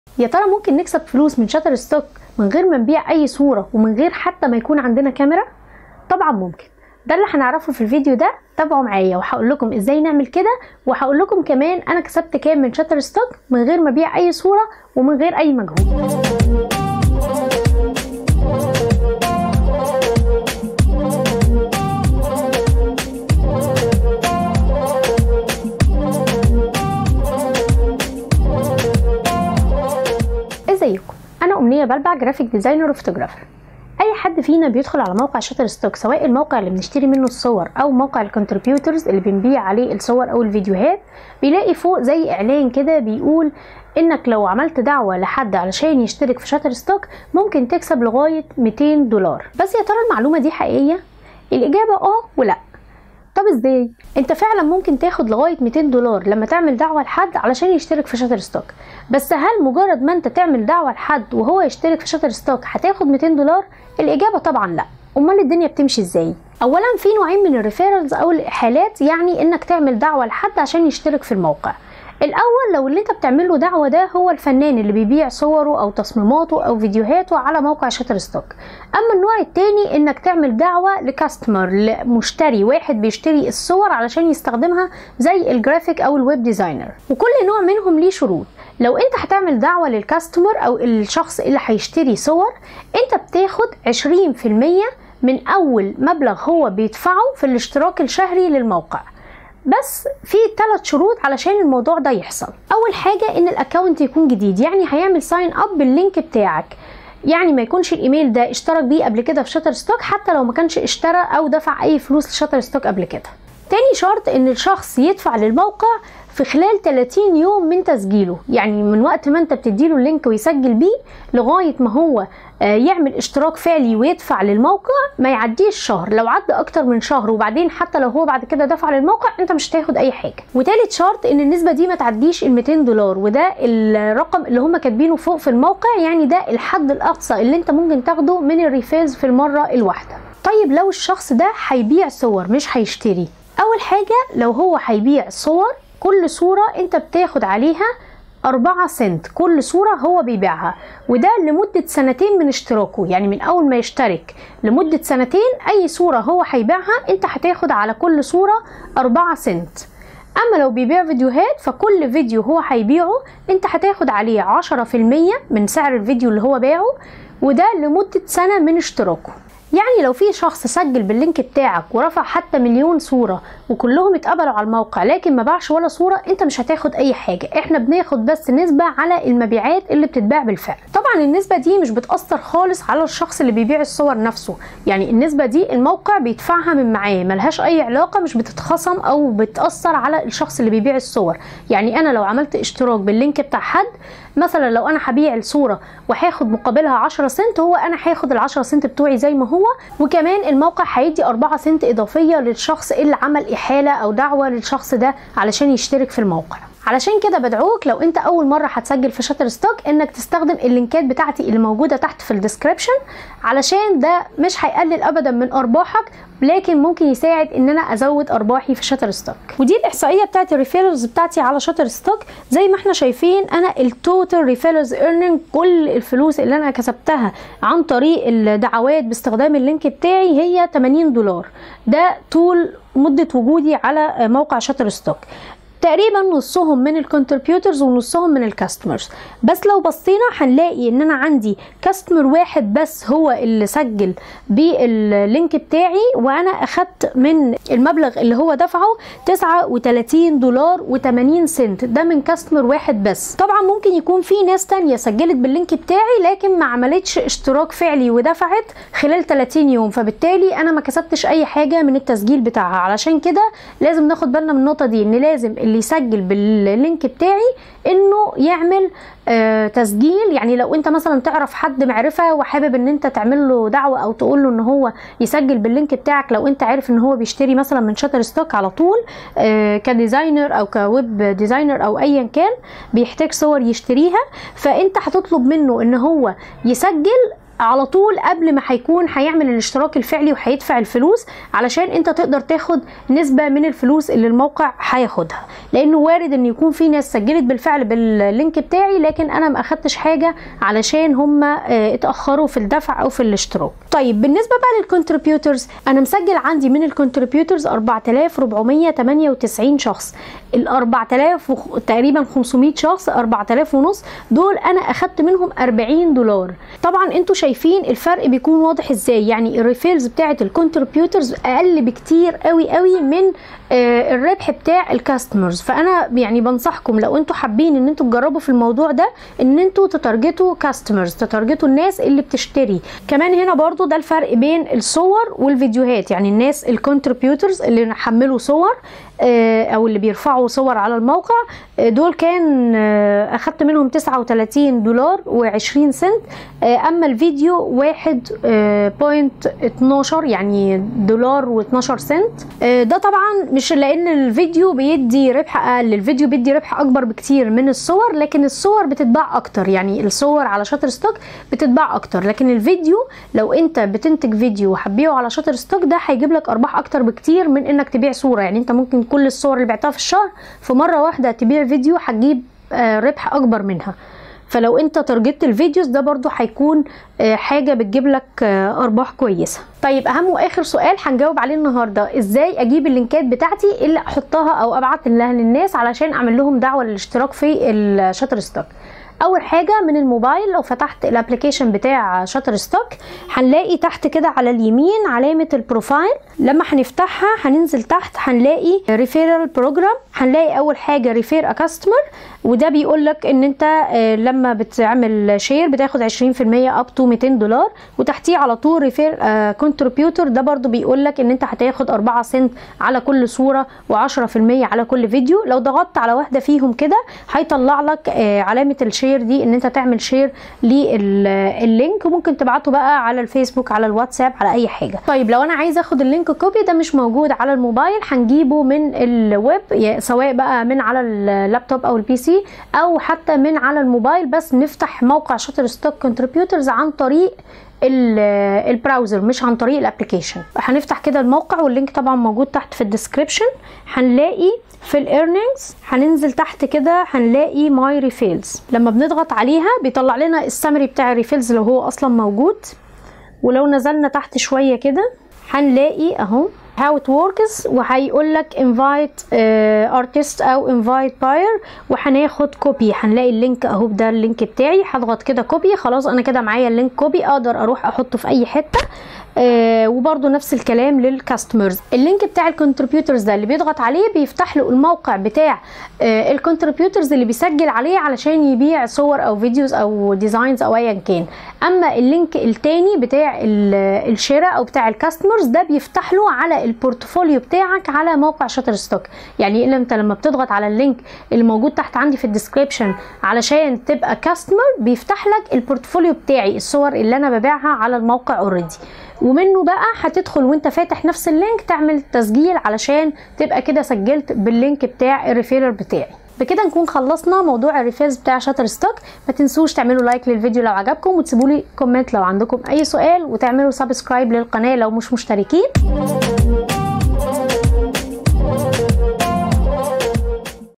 إزاي نكسب فلوس من شاترستوك بدون رفع أي محتوى | الإحالات Referrals
رابط التسجيل في الموقع
لبيع الصور - التصميمات - الفيديو - الموسيقى